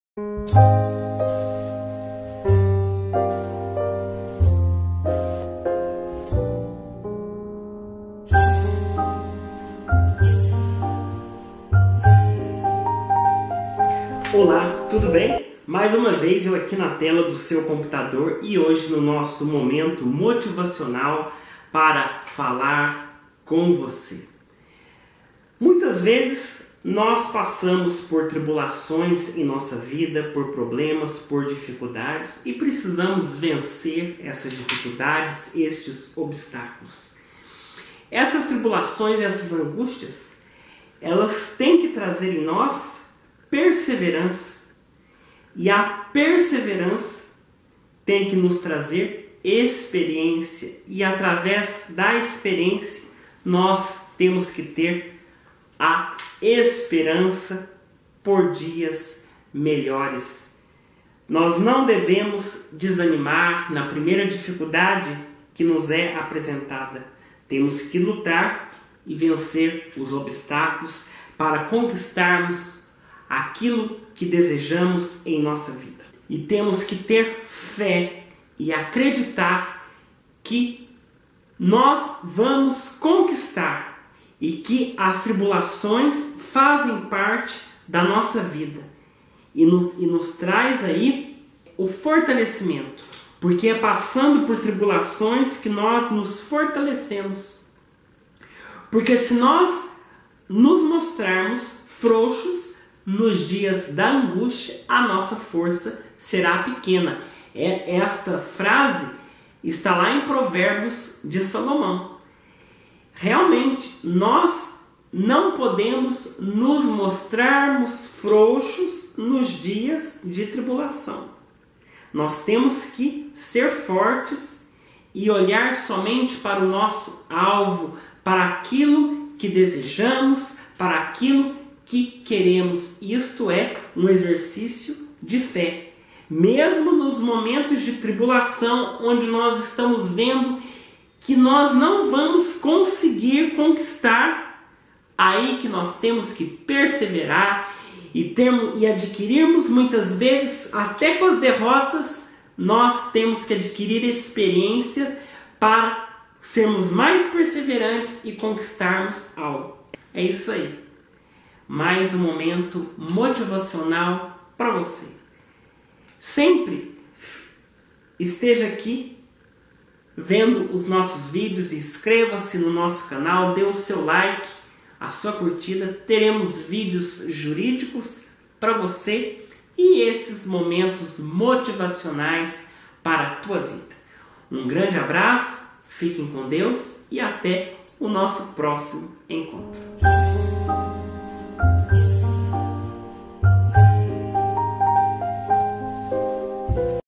Momento Motivacional: DAS TRIBULAÇÕES AO FORTALECIMENTO
É necessário um exercício de fé para o nosso fortalecimento. Uma das receitas está aqui.